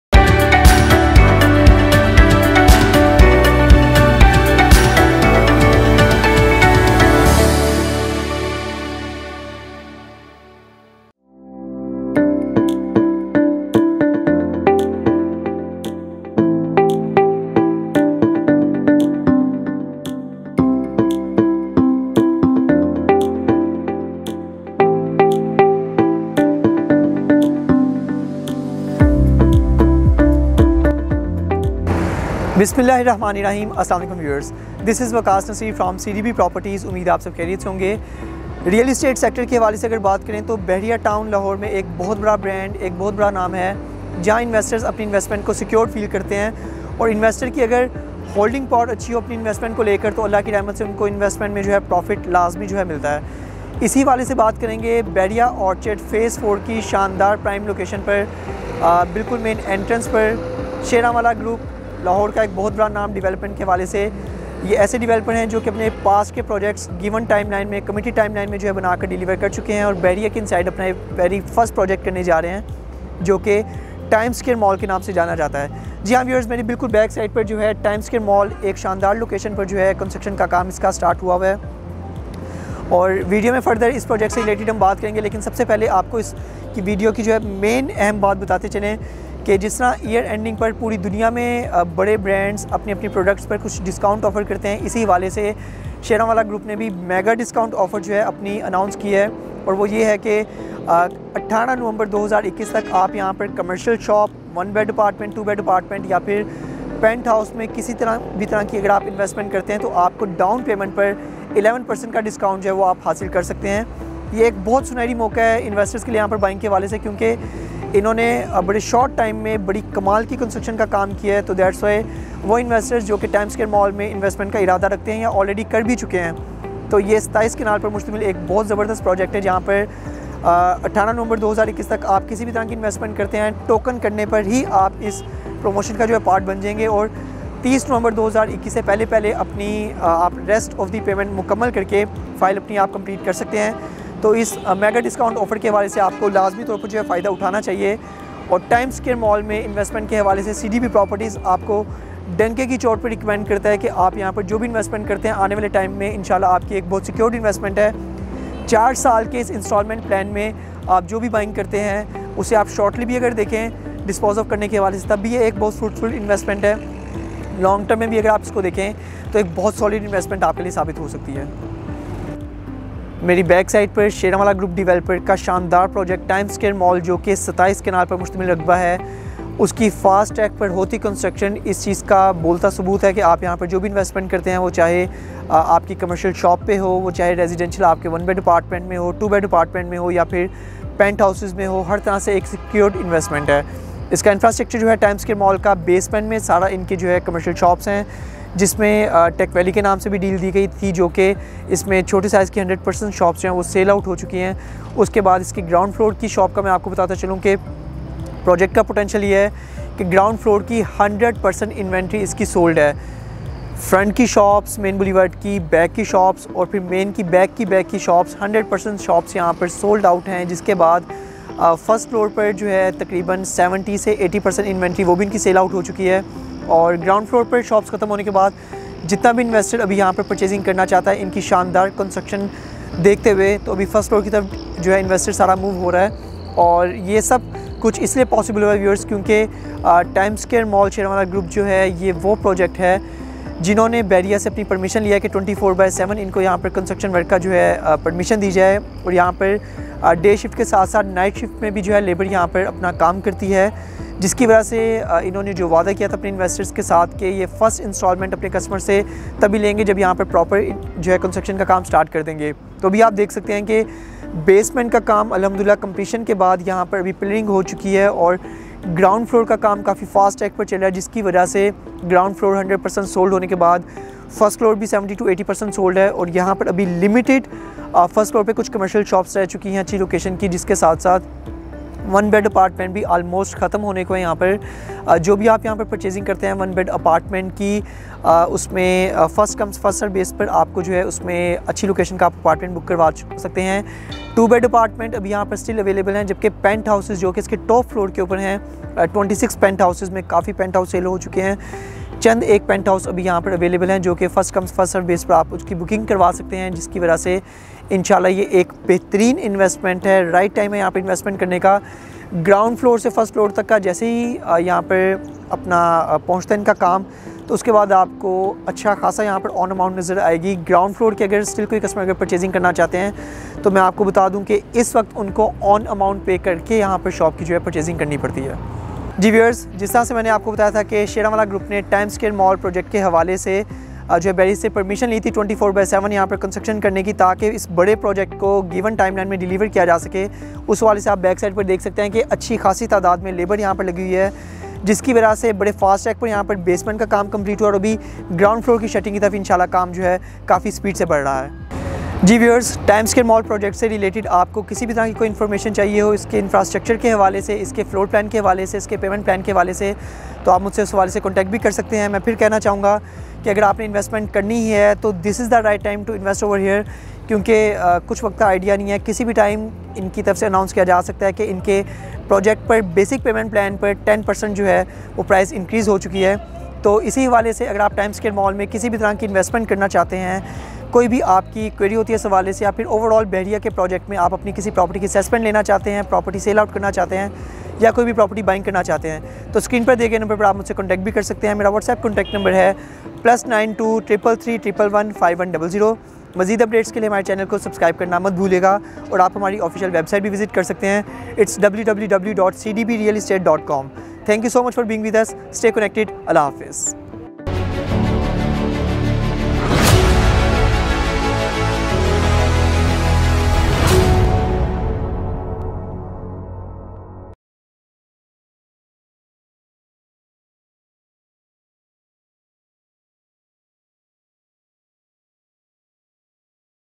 Times Square Mall & Residencia | Perfect Investment | Luxury | Unique | Bahria Orchard Lahore | 2021
𝐓𝐢𝐦𝐞𝐬 𝐒𝐪𝐮𝐚𝐫𝐞 𝐌𝐚𝐥𝐥 & 𝐑𝐞𝐬𝐢𝐝𝐞𝐧𝐜𝐢𝐚  is an ultra-commercial and residential project designed and developed by the Sheranwala Group. Primely located in the vicinity of Bahria Orchard Lahore, Located at a prestigious location with easy accessibility from all parts of the city. 𝐓𝐢𝐦𝐞𝐬 𝐒𝐪𝐮𝐚𝐫𝐞 𝐌𝐚𝐥𝐥 & 𝐑𝐞𝐬𝐢𝐝𝐞𝐧𝐜𝐢𝐚 promises to offer beautiful ultra-luxury apartments with high-class amenities.

BEMC
Lake City
Al-Kabir Town
Alnoor Orchard